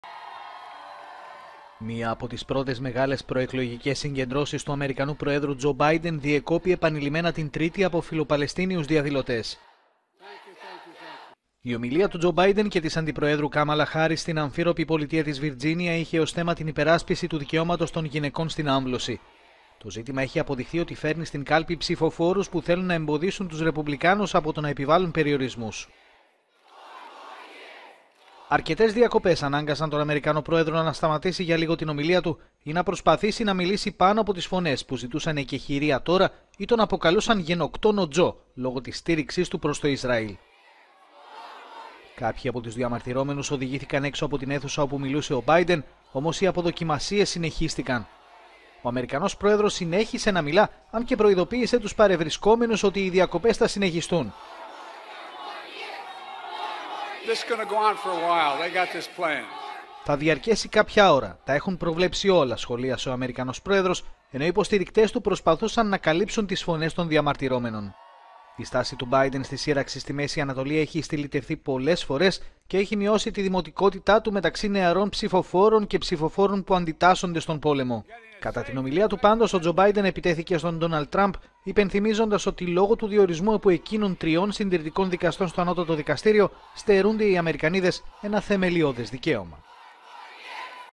ΗΠΑ: Φιλοπαλαιστίνιοι διαδηλωτές διέκοψαν ομιλία του Τζο Μπάιντεν για την άμβλωση
Κάποιοι αποκάλεσαν τον Αμερικανό πρόεδρο «γενοκτόνο Τζο», στηλιτεύοντας την στήριξή του προς το Ισραήλ και τον Μπέντζαμιν Νετανιάχου.